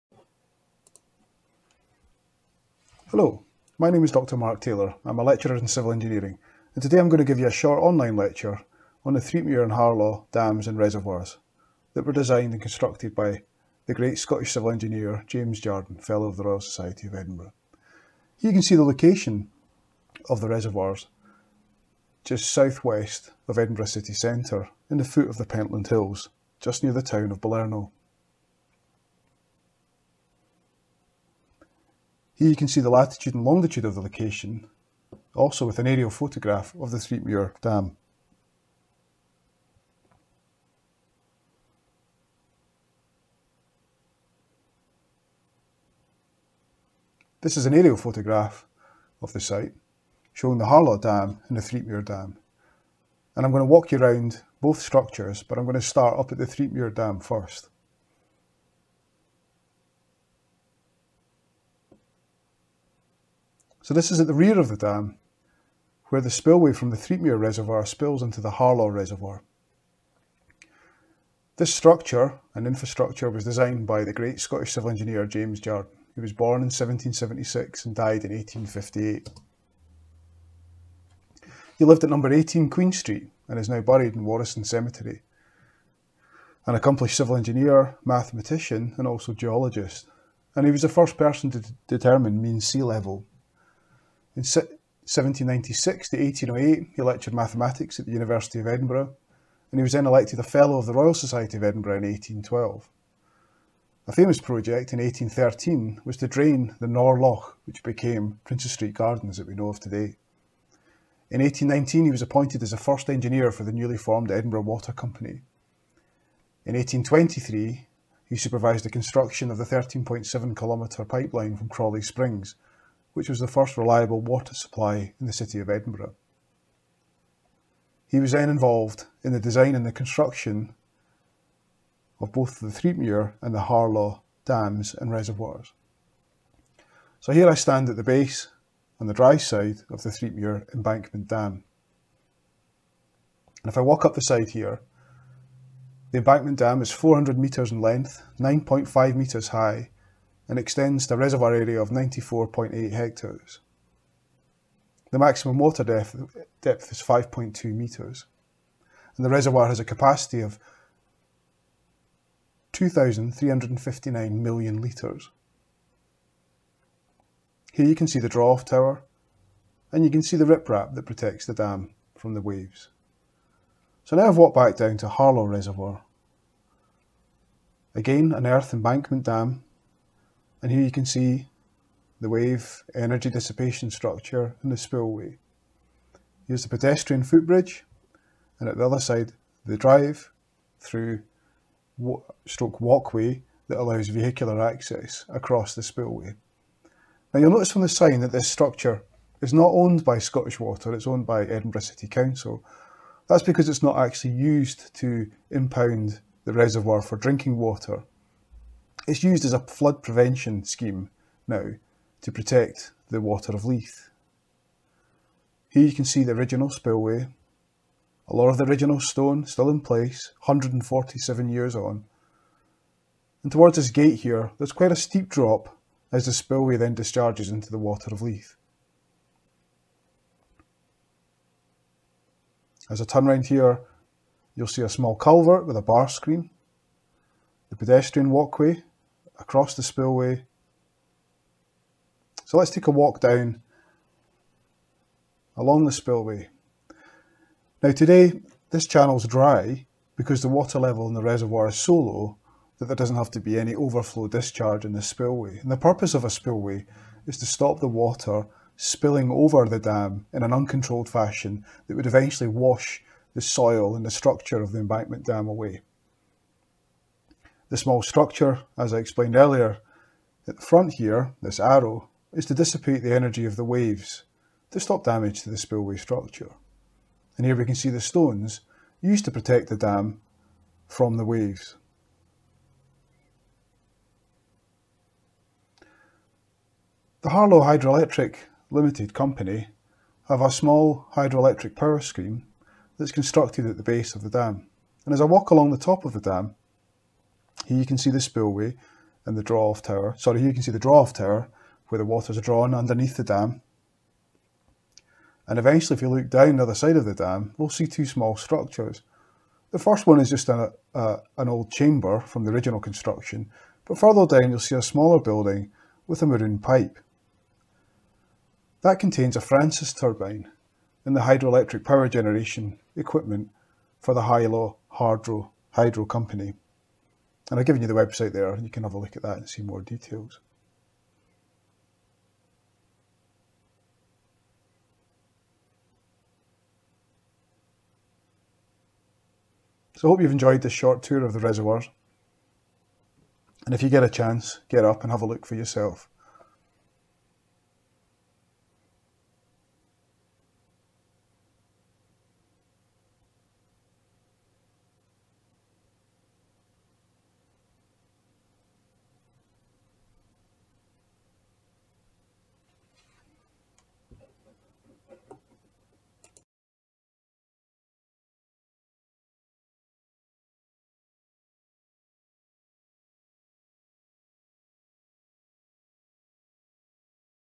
Threipmuir and Harlaw Embankment Dams
A short lecture on the work of James Jardine FRSE and the construction of the earth embankment dams, reservoirs and spillways.